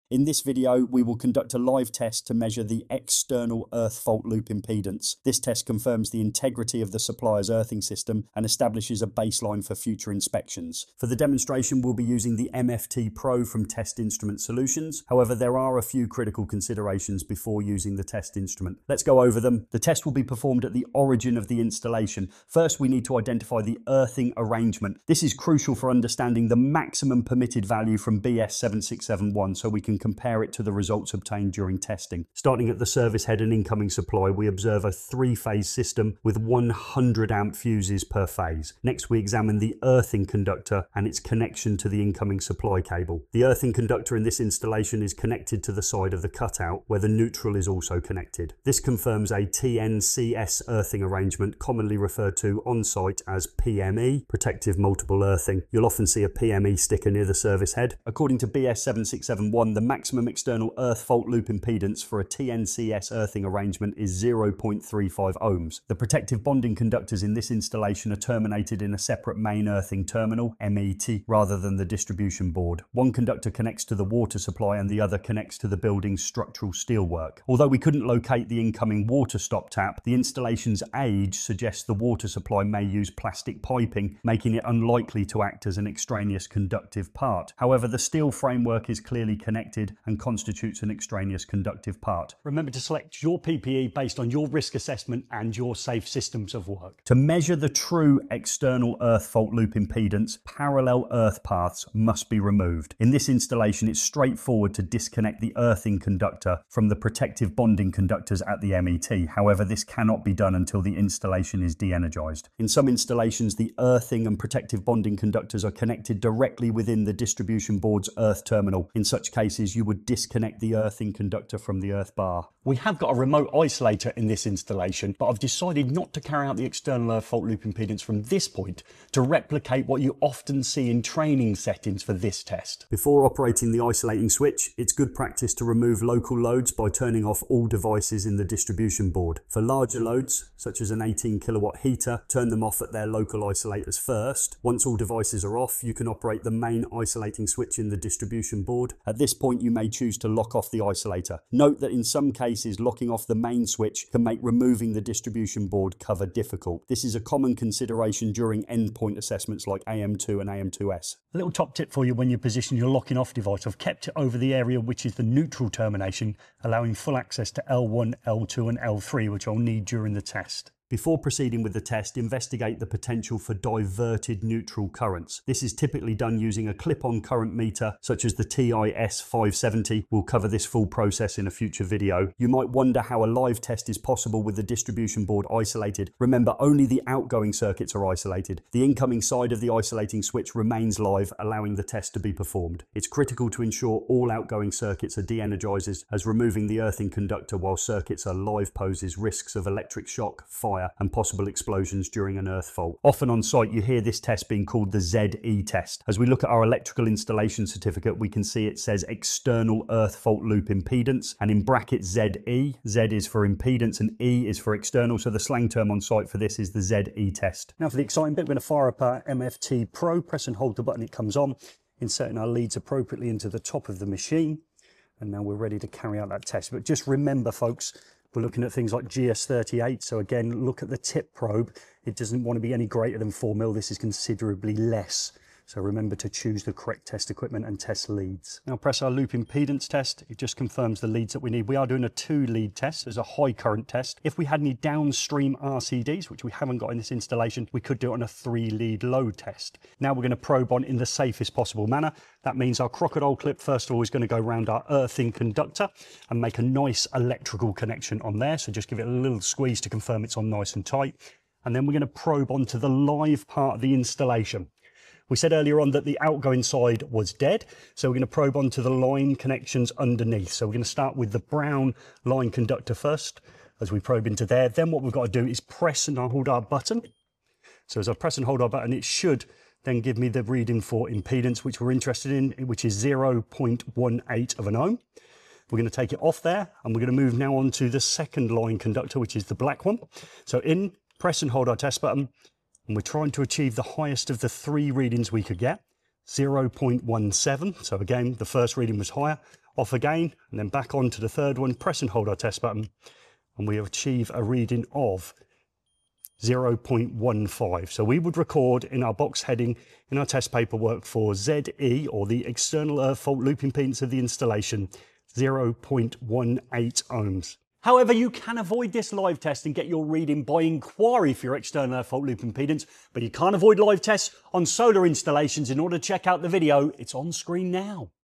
External Earth Fault Loop Impedance Testing Explained (Ze)
This video demonstrates external earth fault loop impedance testing using the MFT Pro from Test Instrument Solutions. This live test confirms the integrity of the supplier's earthing system and sets a baseline for future inspections. Before testing, we’ll cover key considerations, including identifying the earthing arrangement, disconnecting parallel earth paths, and ensuring safety protocols are in place. Watch as we perform the test step-by-step, explain critical readings, and highlight common on-site practices like the Ze test. Perfect for anyone looking to improve their fault loop impedance testing skills!